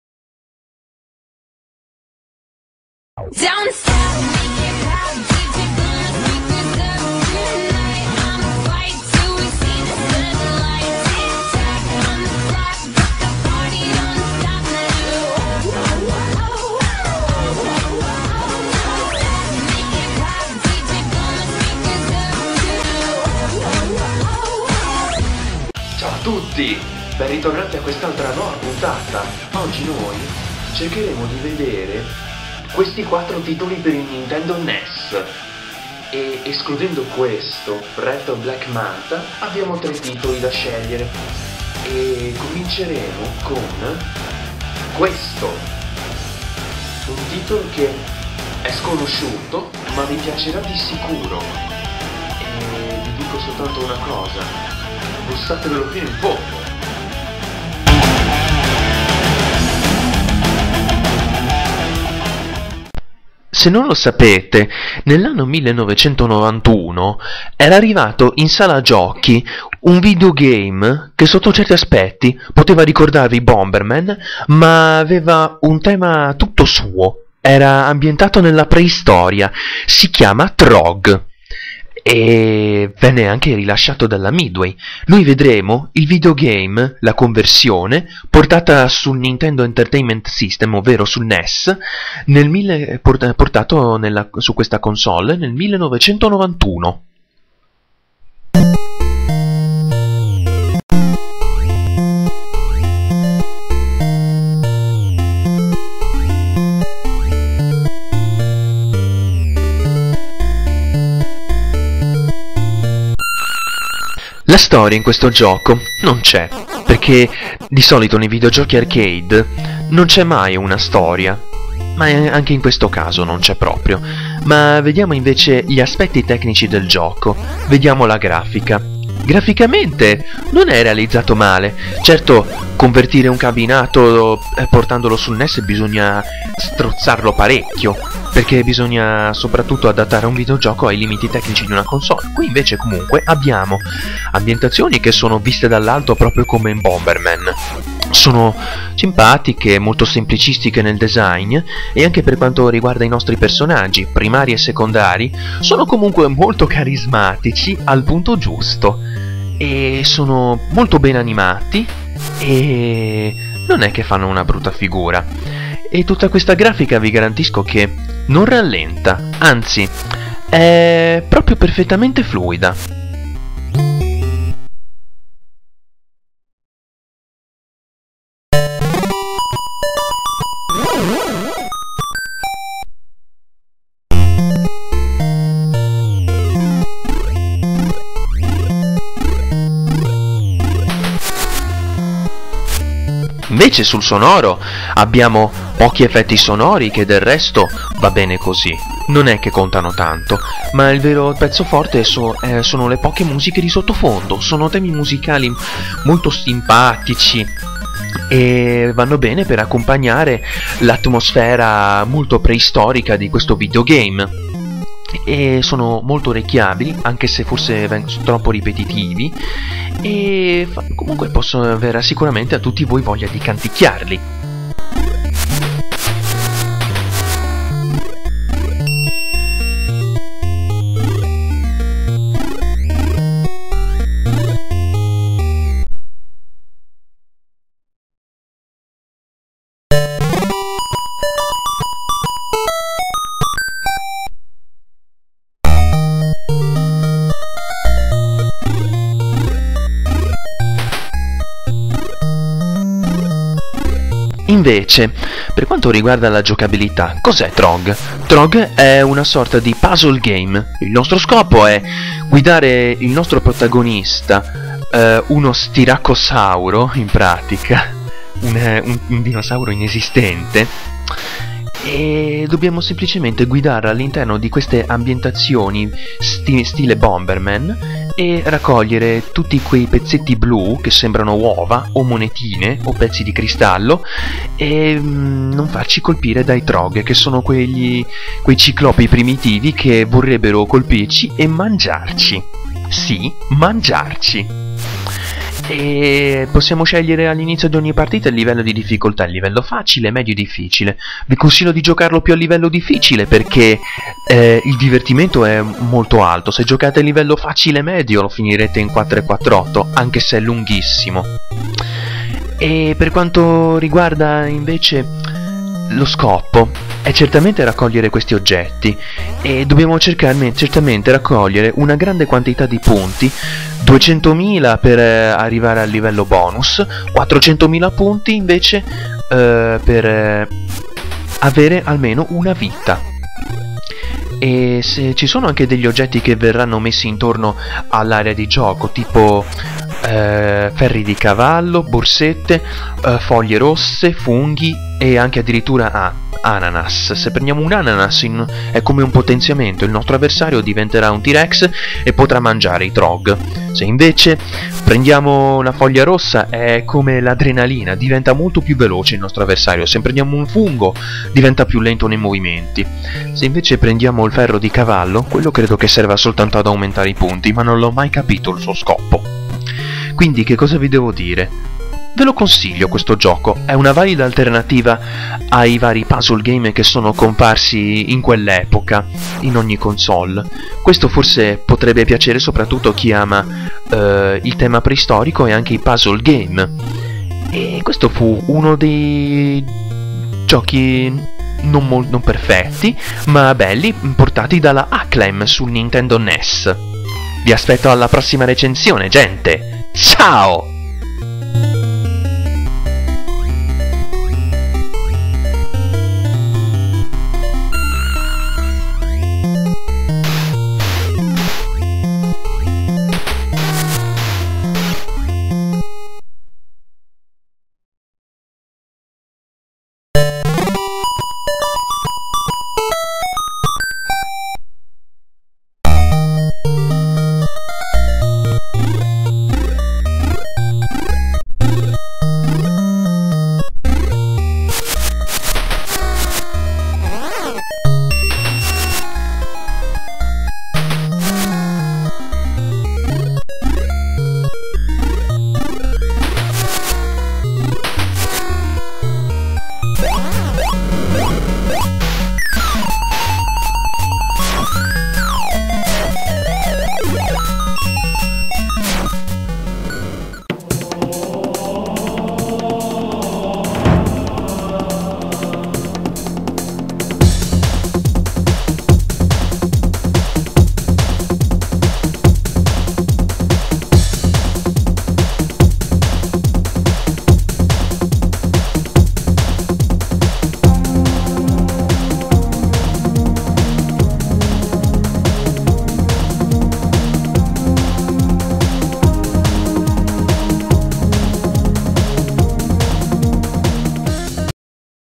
Recensione Trog NES
Oggi torniamo a vedere un altro gioco per la console Nintendo NES, e vediamo un gioco rilasciato dalla Acclaim che allora era una casa che contava e realizzava giochi di grande classe.
Questo gioco non è un titolo completamente originale ma una consversione di un cabinato, tra l'altro è ben riuscita e il divertimento non manca. Provatelo e dategli una possibilità.